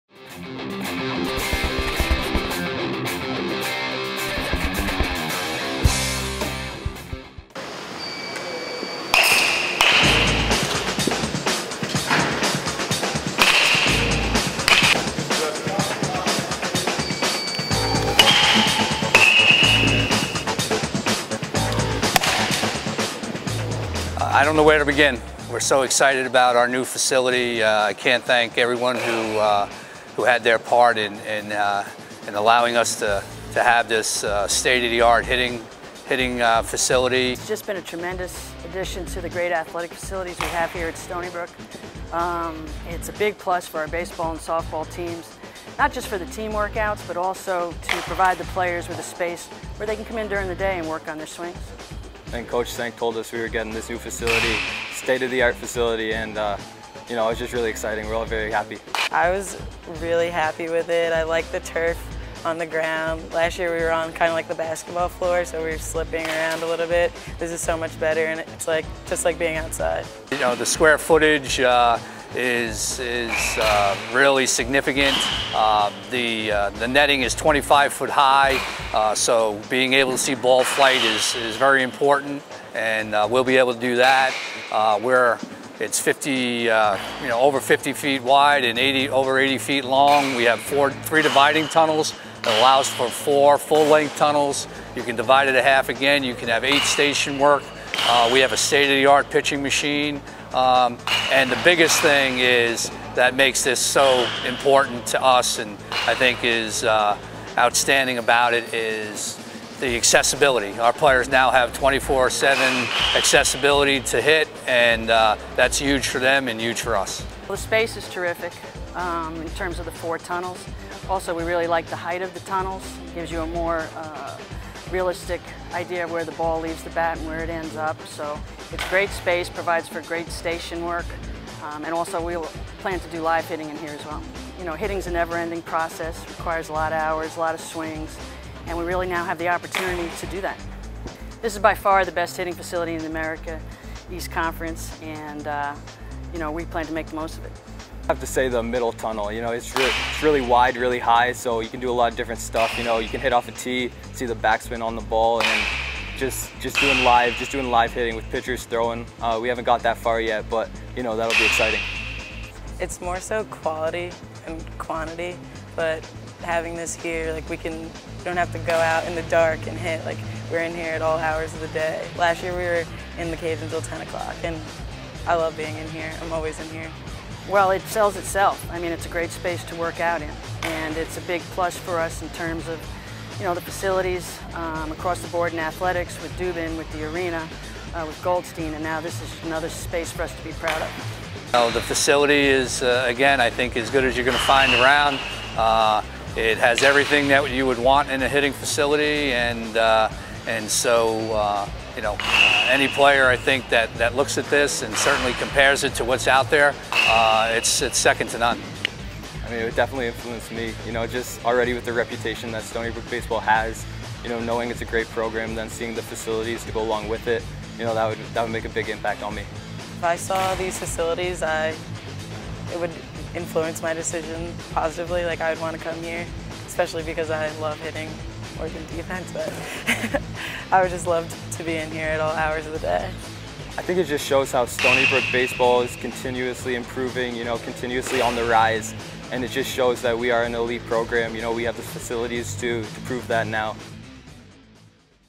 Stony Brook Athletics unveils the Seawolves Hitting Facility
Stony Brook Athletics has unveiled its new Seawolves Hitting Facility, a 5,000-square foot space that will serve the indoor practice needs of the baseball and softball teams.